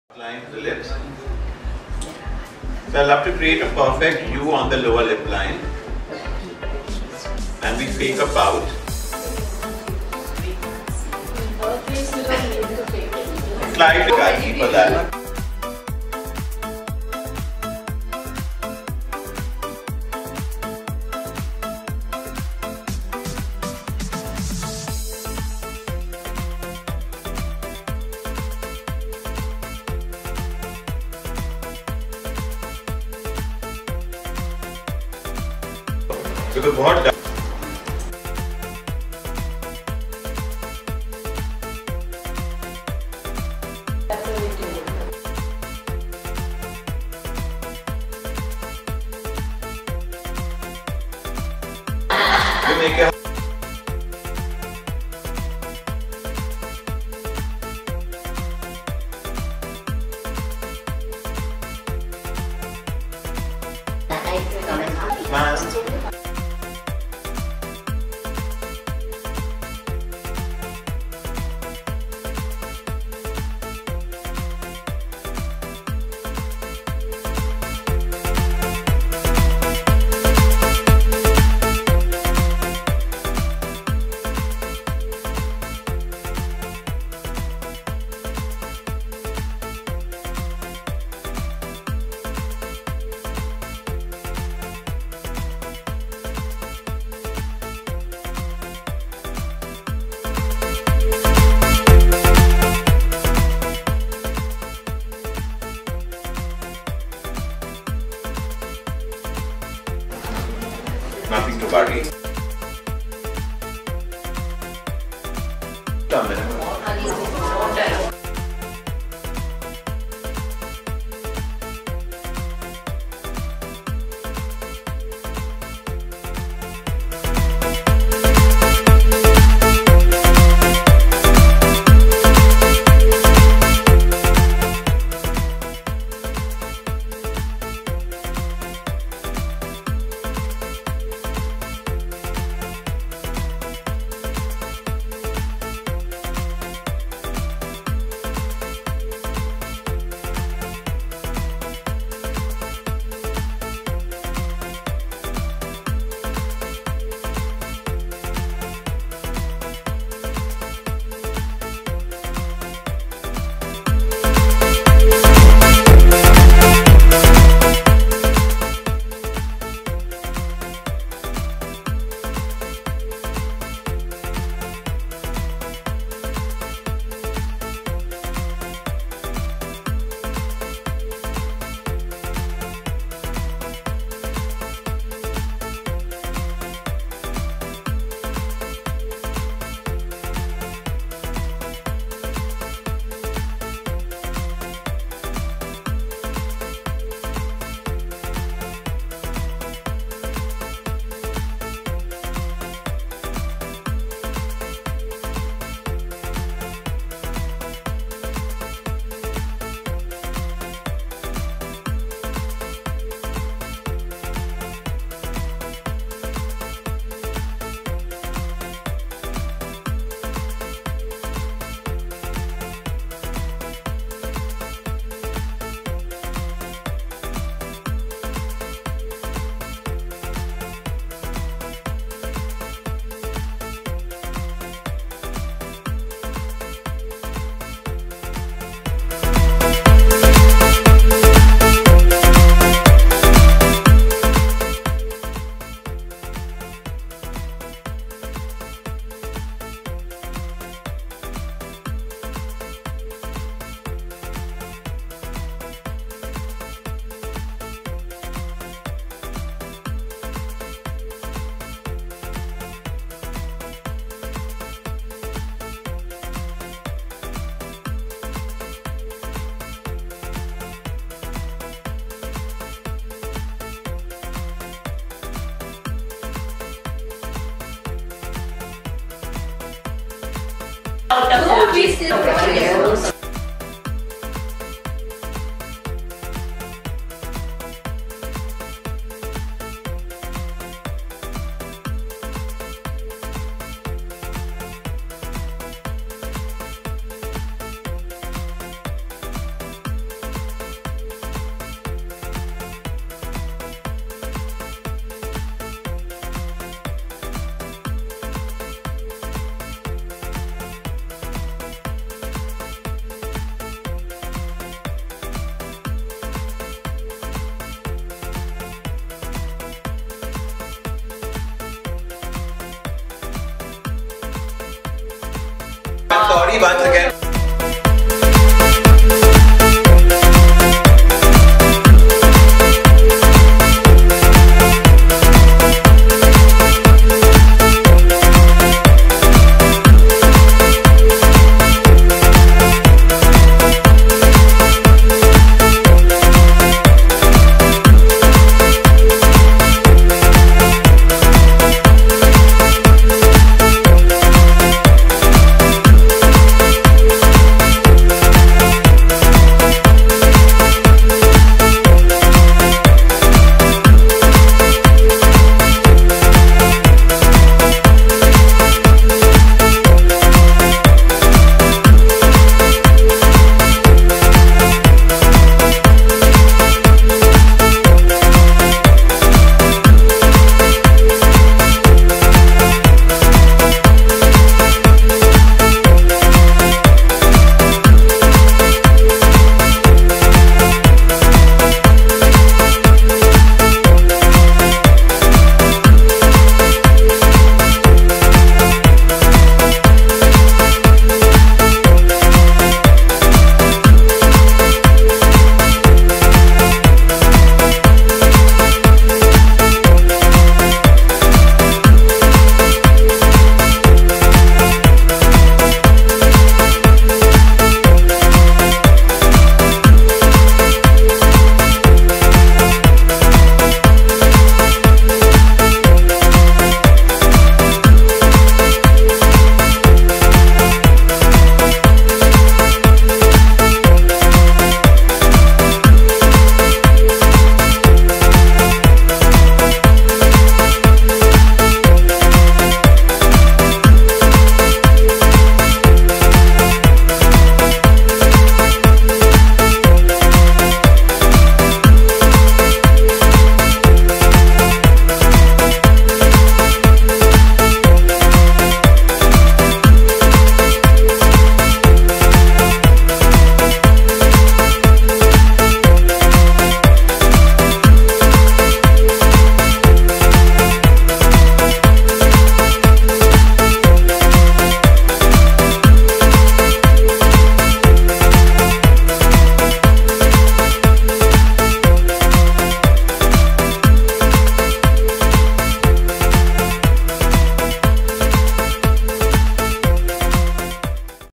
Episode 03 | Season 6 |Makeup & Hair Tutorial | Lip Makeup | Hairstyle Tutorial
Let's look at the stunning Ojas Rajani focusing on the lips, making it look so confident and vibrant.
Bombay Pier District 3141
Prisim- The Healing Studio
Rotract Club of Bombay
Transformation Salon

Makeup & Hair: Ojas Rajani
Jewellery: Ojas Rajani
Outfit: Aanchals